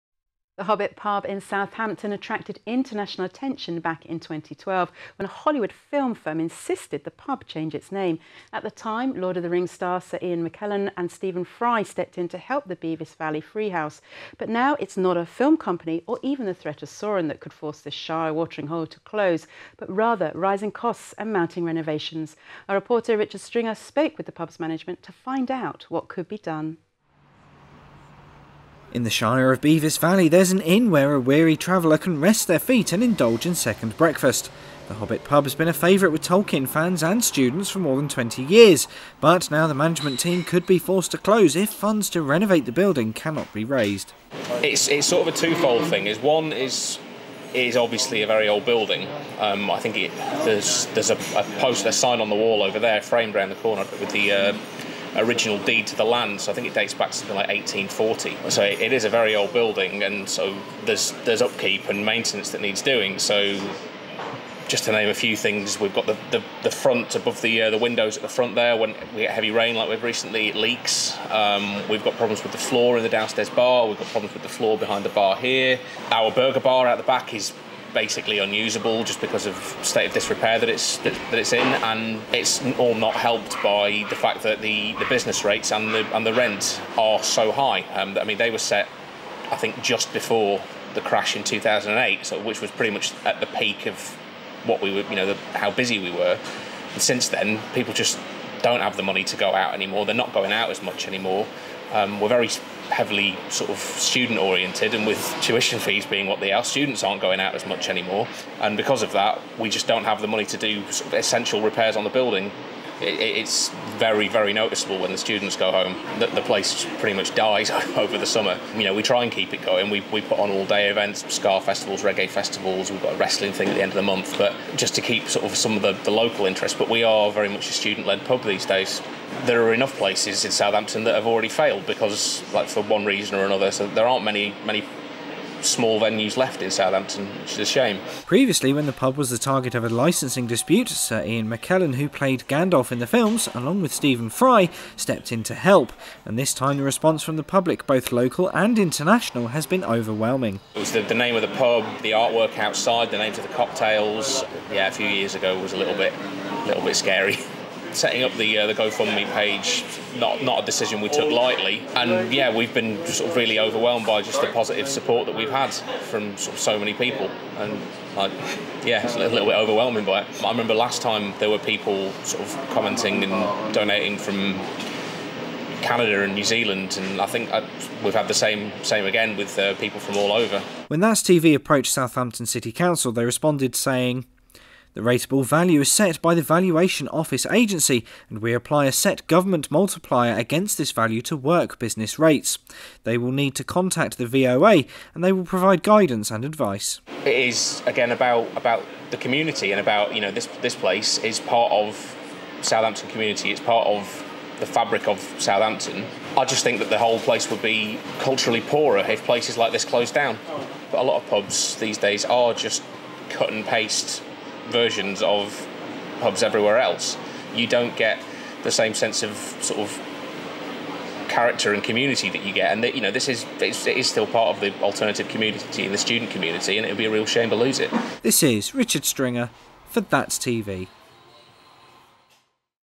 A Tolkien Themed Pub In Southampton Faces Closure
The Hobbit pub in Southampton attracted international attention back in 2012 when a Hollywood film firm insisted the pub change its name.
 At the time Lord of the rings star Sir Ian McKellen and Stephen Fry stepped in to help the Bevois valley free house.
 But now it’s not a film company or even the threat of Sauron that could force this Shire watering hole to close, but rather rising costs and mounting renovations.